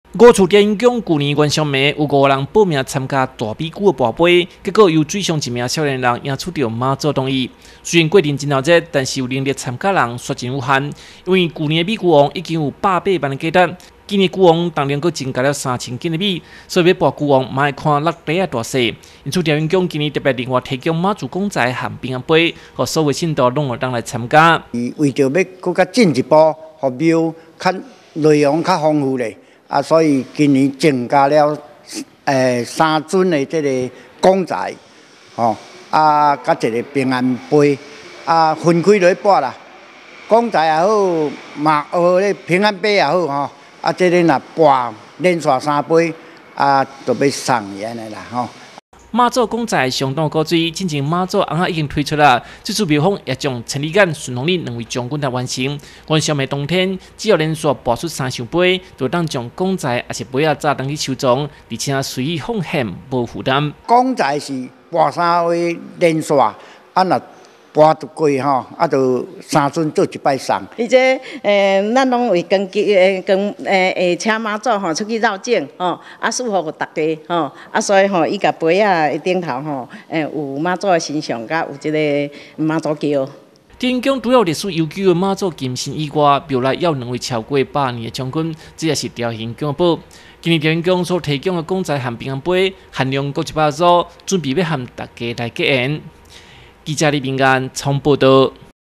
雲林新聞網-朝興宮慶元宵 送公仔.平安杯
西螺朝興宮的大米龜，今年已經來到六萬七千一百六十八斤，換算成金額，大約有兩百萬的價值，所以大米龜有很多人看，但不是人人都敢去擲筊，因此廟方今年特別提供了限量的媽祖公仔和平安杯，讓每一個信徒都能來求。